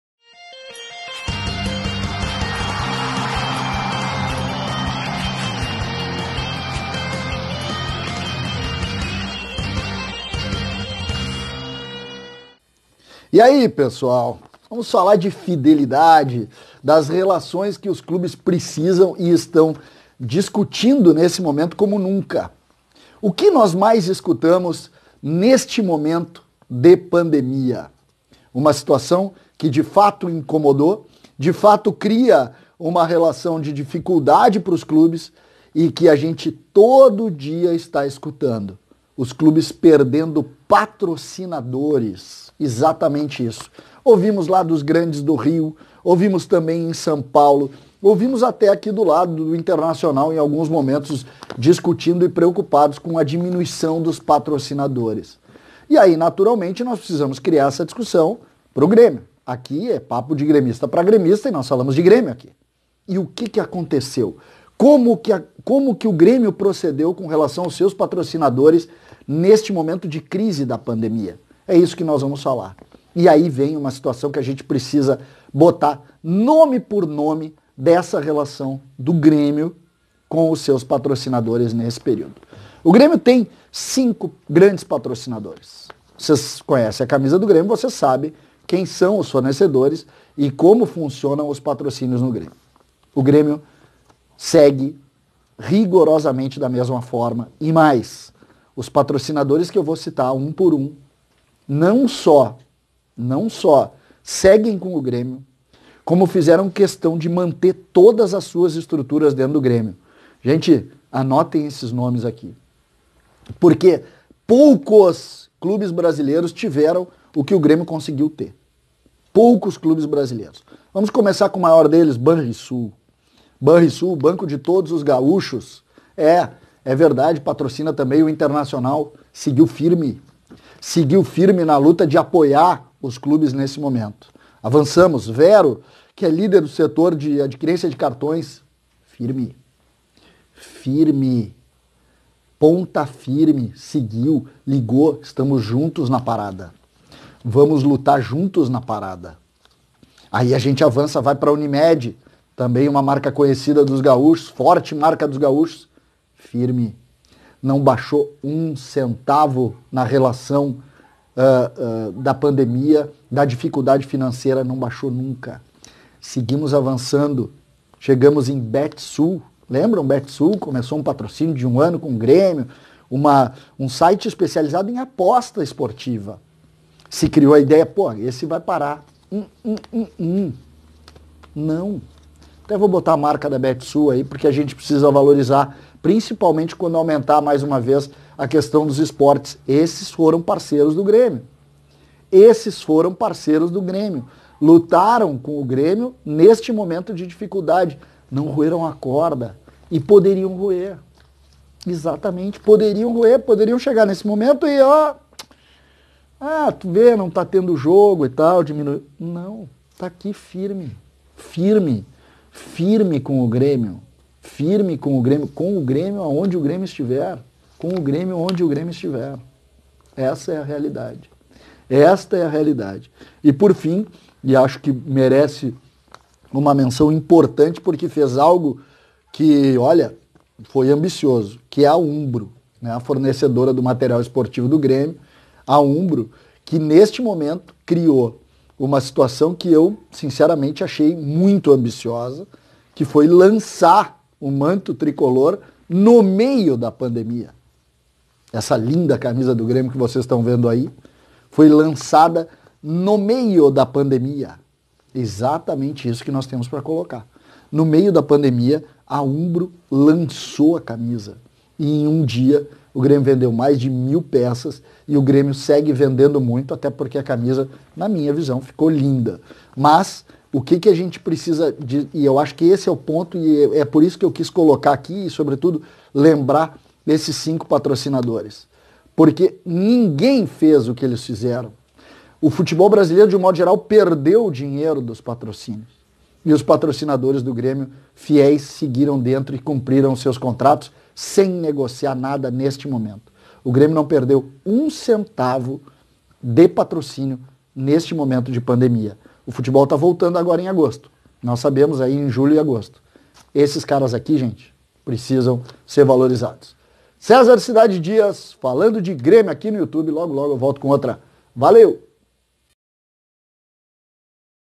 A verdade sobre patrocínios no Grêmio e no futebol brasileiro na pandemia. Quem saiu e quem ficou
Muito se fala sobre a dificuldade financeira dos clubes em tempos de pandemia. Os patrocínios, segundo muitas matérias, tem deixado os clubes de futebol.